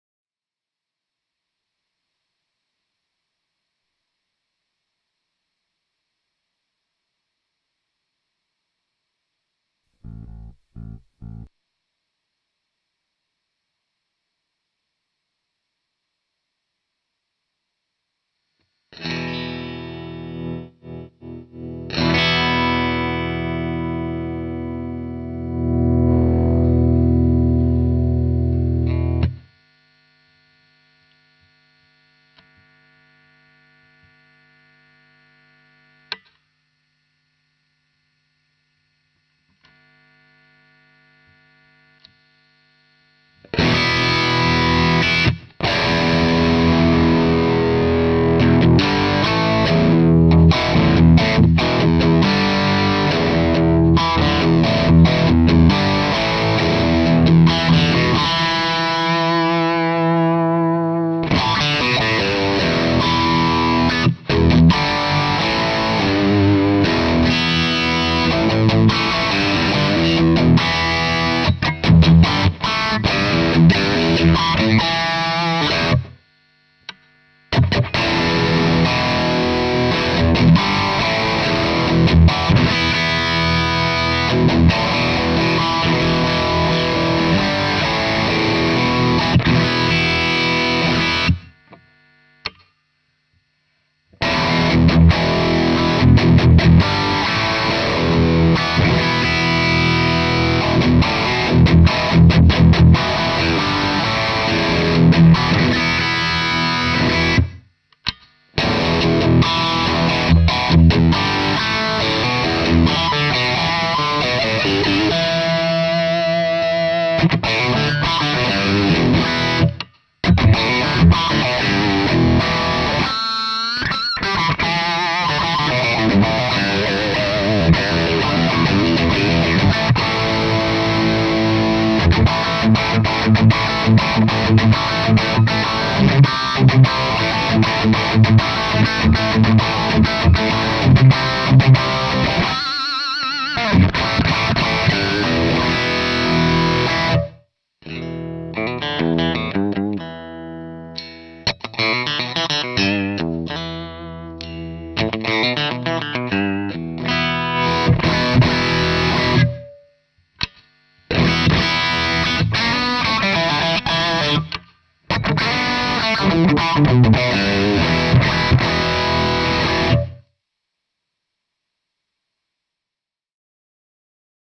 AGL Hiddenbucker Preview One
Short first preview of another brand new AGL thing soon to come! The AGL Hiddenbucker wiring!

Turn it loud, cheers, ACY!